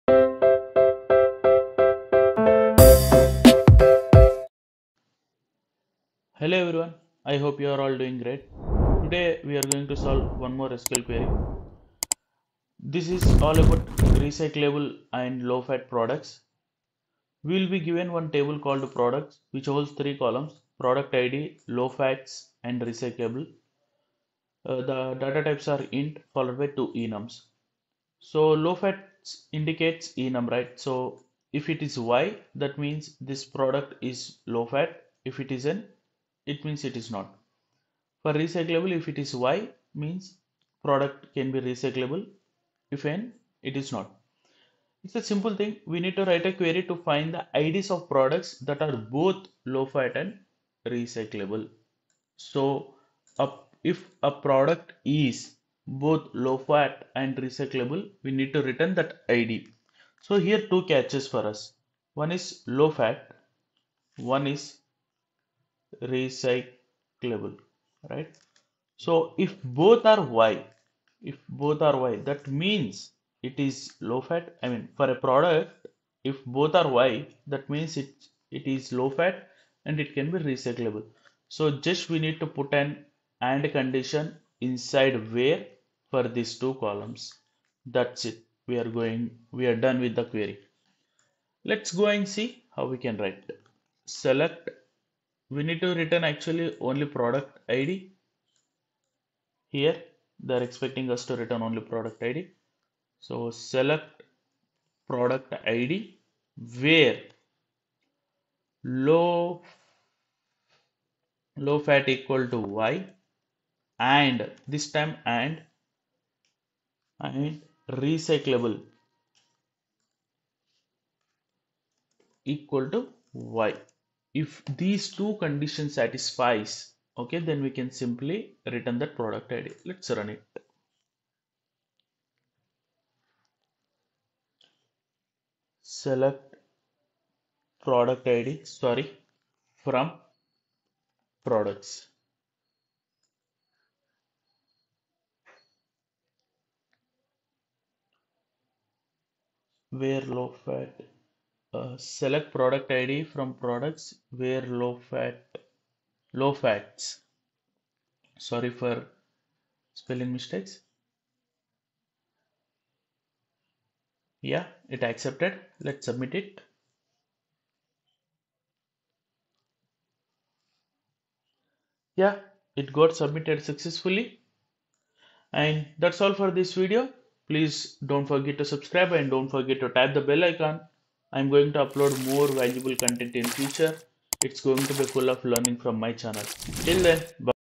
Recyclable and Low Fat Products | Leetcode 1757 | SQL Series
Hey Everyone,

This is second video on SQL Series, going forward I'm going to cover complex queries along with detailed explanation.

Note : All uploaded content in this channel is mine and its not copied from any community , you are free to use source code from above mentioned GitHub account.